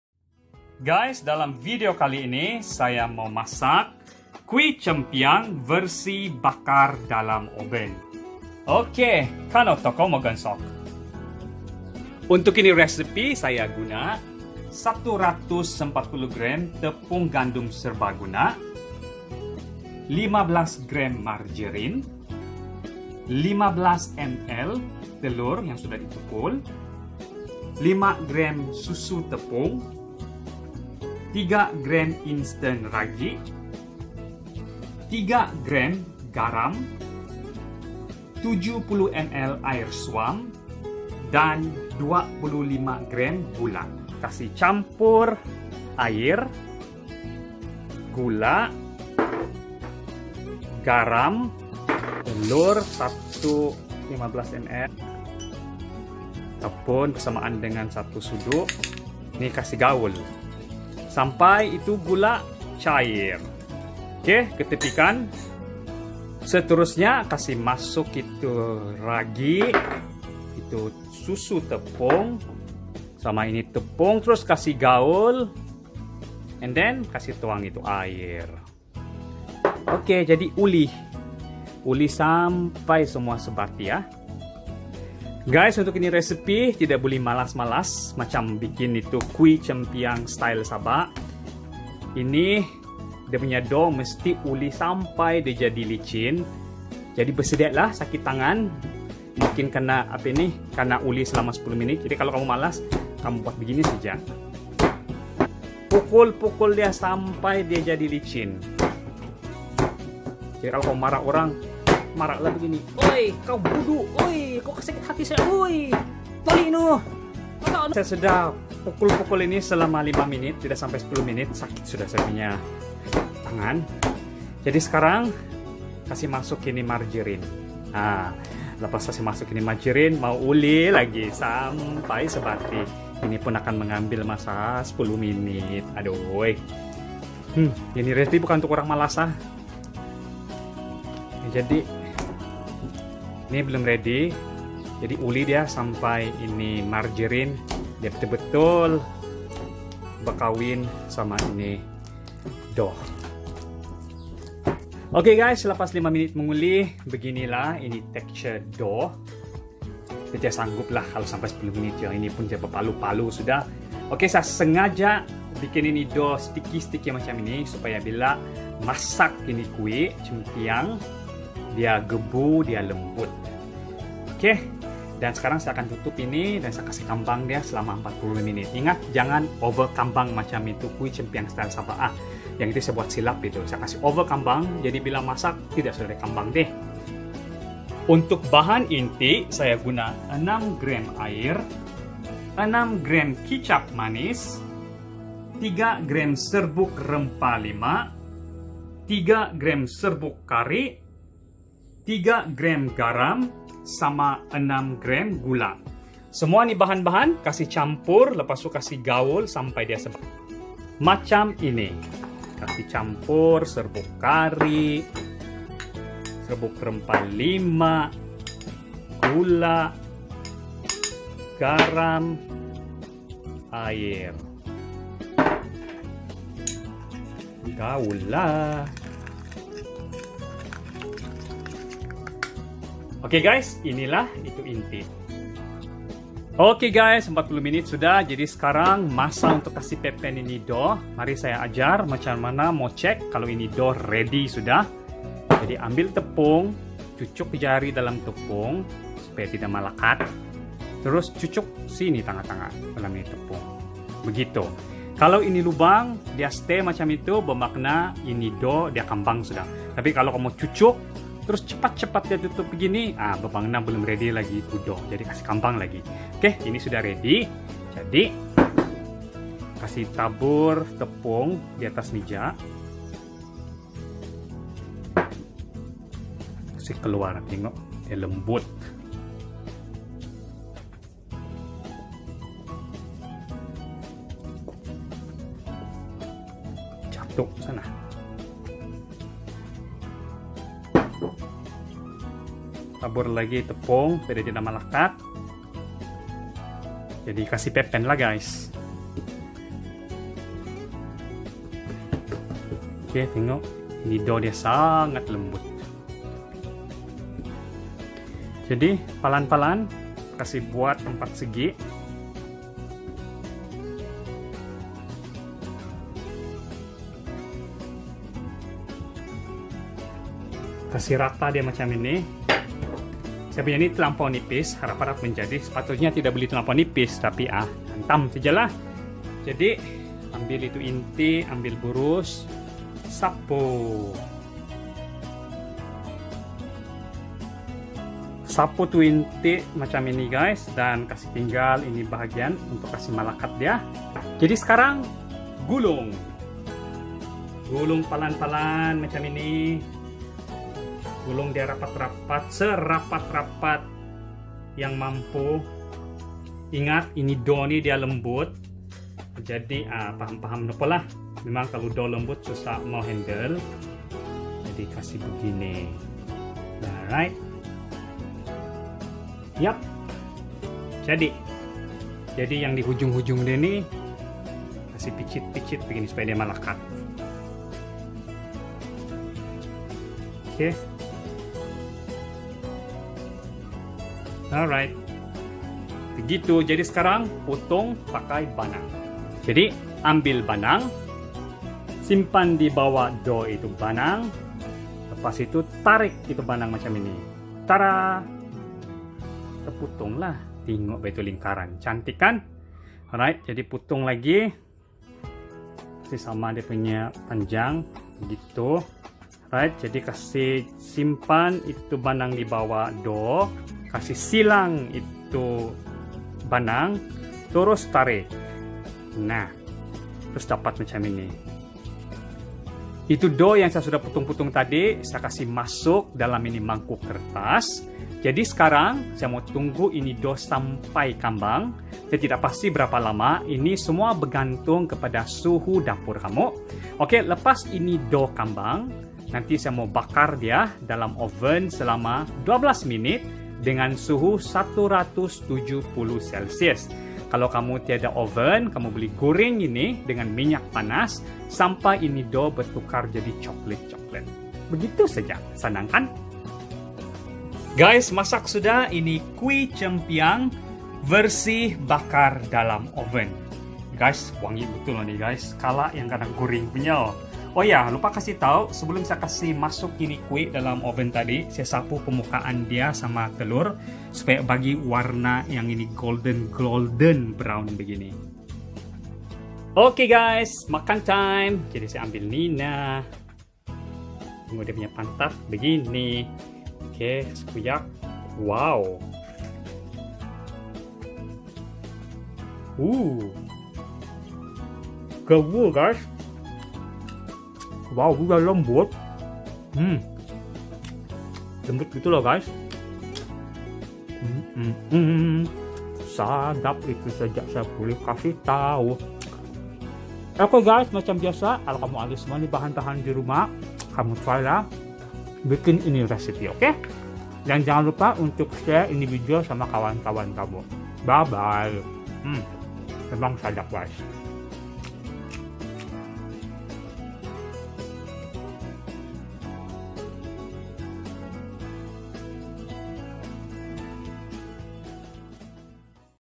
Resipi Cempiang Bakar Dalam Oven @ cinnamon rolls inti Cempiang Sabah
Resipi Kuih Cempiang Bakar Dalam Oven

Nah guys Resipi Kuih Cempiang Sabah versi bakar dalam oven. Ini resipi untuk yang rajin dan ada tahap kesabaran tinggi. Kalau pamalas, tingu video jak la ah. 😂

Ini versi Kuih cempiang sangat lembut dan gebu. Ini nda semestinya bakar dalam oven jak ah. Kasih guring pun buli ni.

Guna ni recipe doh untuk masak bun yang ada inti. Macam bun tausa, bun kaya atau pun bikin tu bun sausage.

Senarai bahan-bahan ada kana tulis di penghujung video.

Selamat mencuba!!